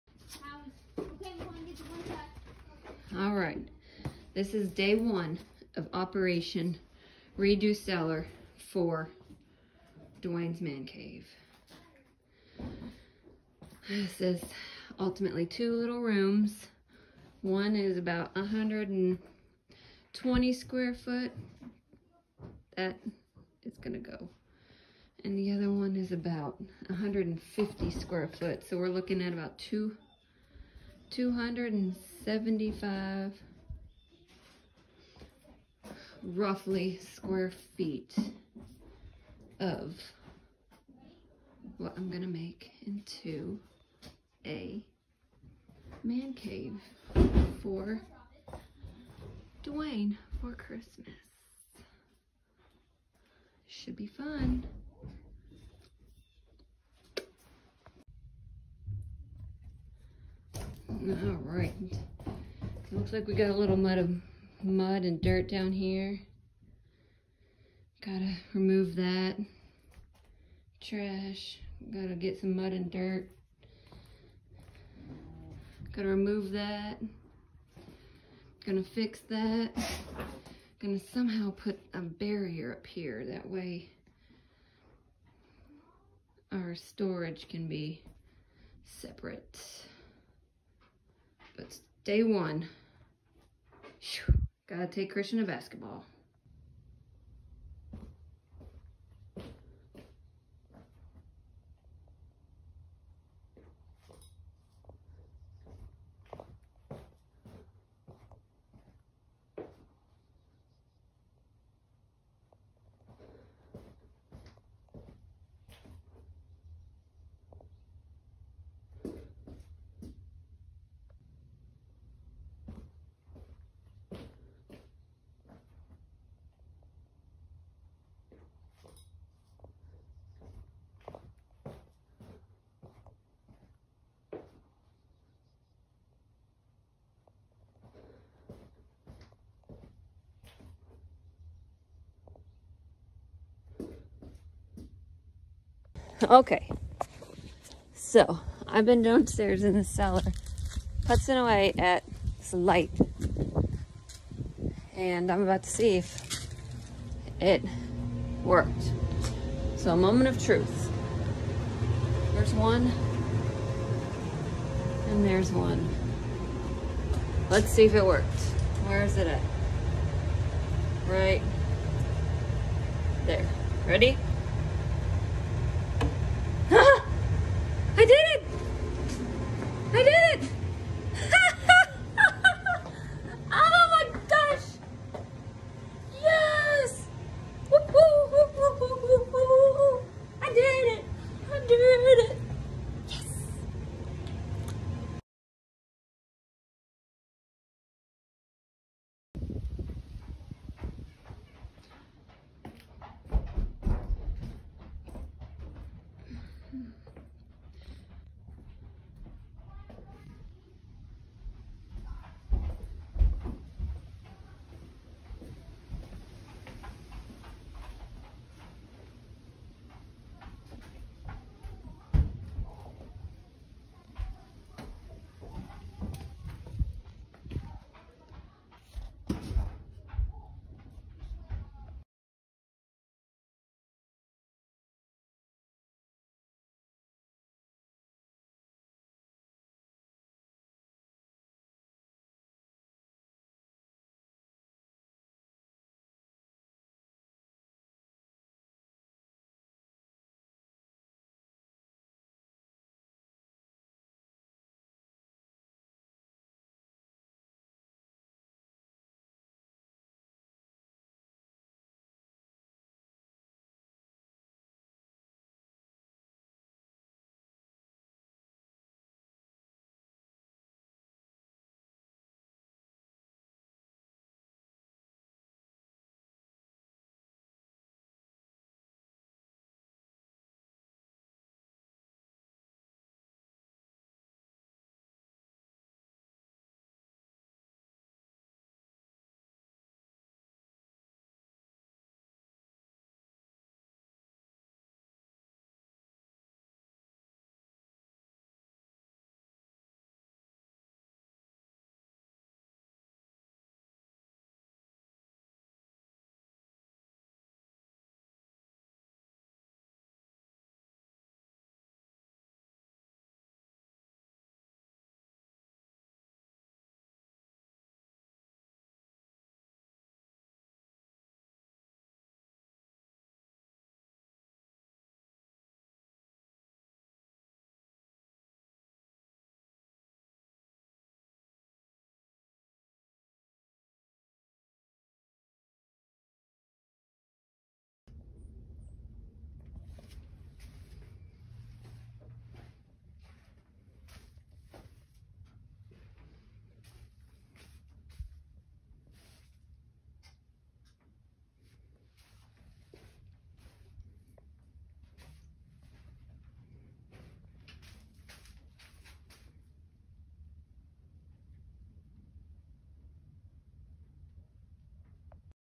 Very quick transformation video of the cellar turned man cave.
This will be more of a game room for kids I’m sure. We’re gonna add slot machines and large arcade games.